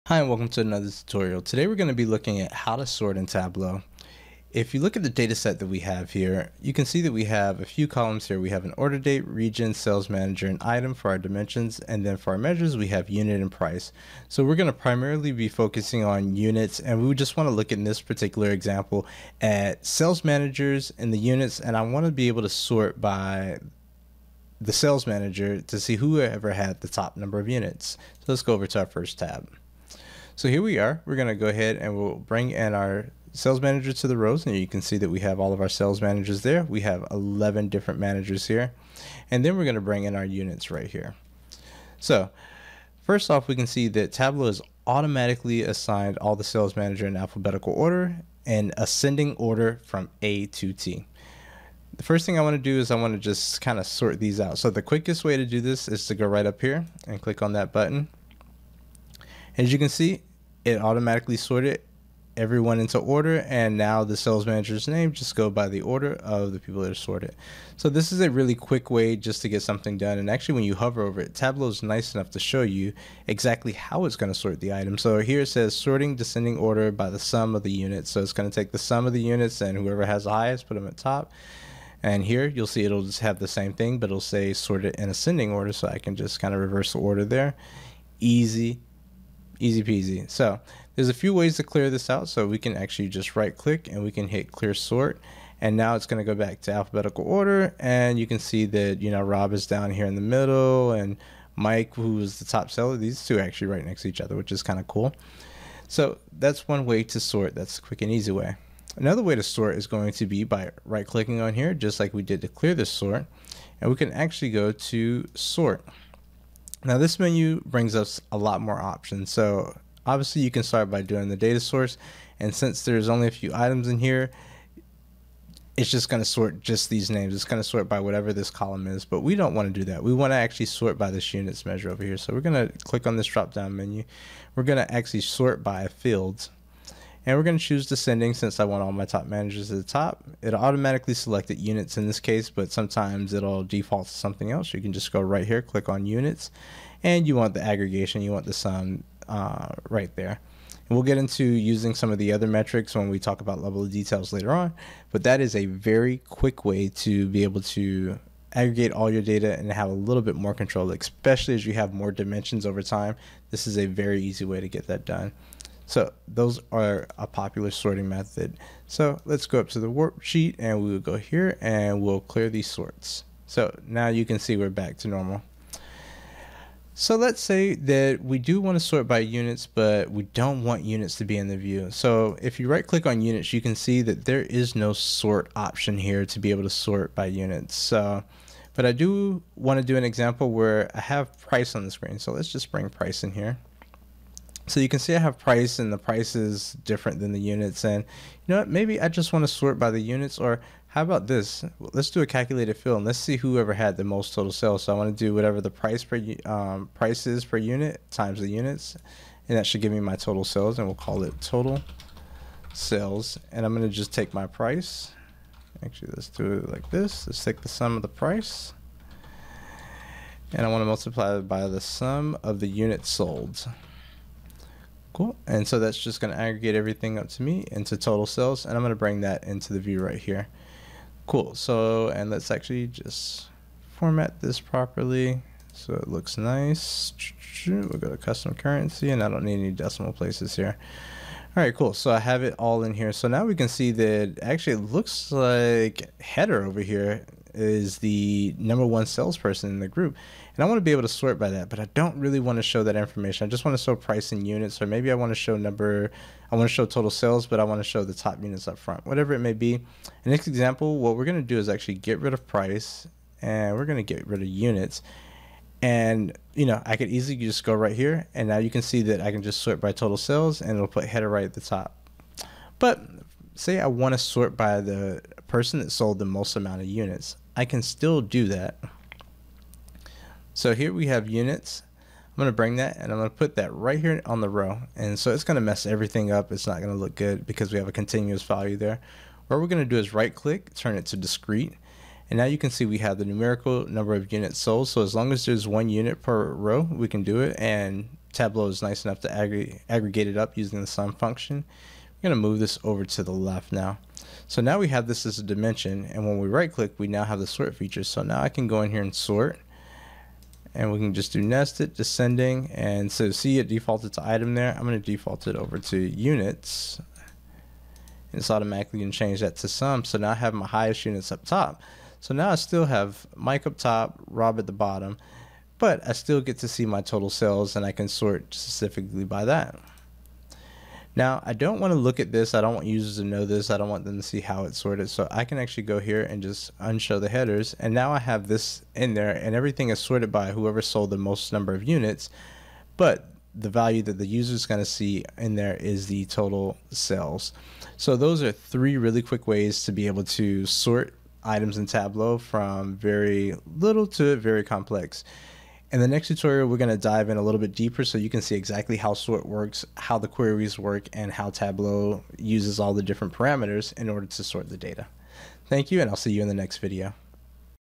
How to Sort in Tableau
There are 3 primary ways to sort in Tableau and this article will cover each of them. We will even show you how to clear you sort after you have made a selection. There 4 parameters to consider in the advanced sort. But the primary thing you need to know is which field to sort and if you want it in Ascending order or Descending order.

1. Go to the toolbar at the top of the page.
2. There are three icons to choose from (Swap Rows & Columns, Sort Descending, Sort Ascending.
3. Click on the way you want to sort.
4. Tableau will automatically sort based on the measure you have in the view